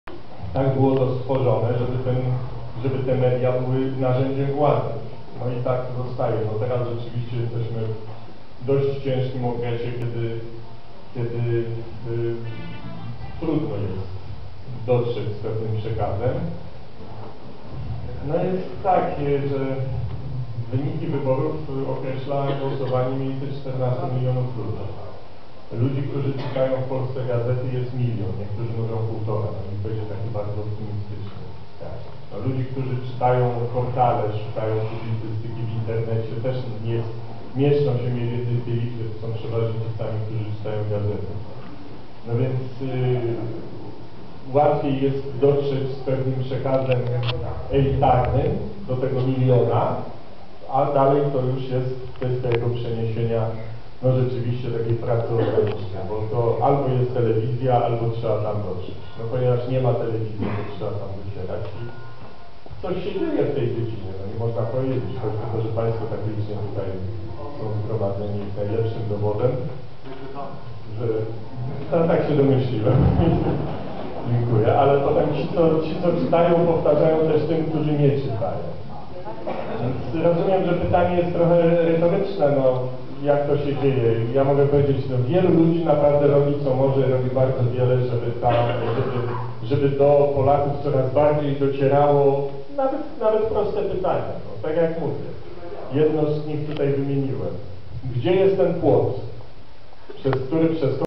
Rafał Ziemkiewicz w Poznaniu podsumowanie wyborów 28.10.2011..avi
Rafał Ziemkiewicz w Poznaniu mówi o minionych wyborach.  Zapraszam do obejrzenia na moim koncie innych wypowiedzi również i Pana Rafała.